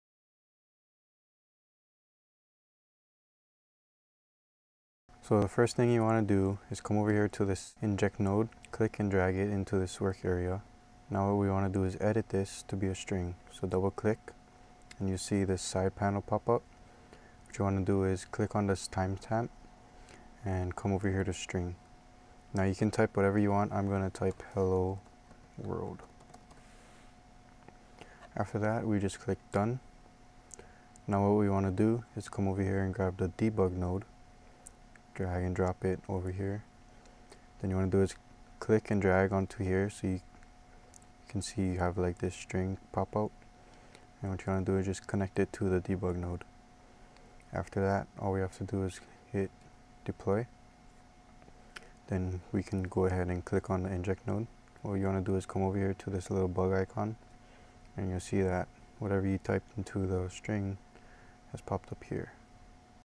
RPi Node-Red: Inject + Debug = Hello World
This video will teach you the basics of the inject and debug nodes.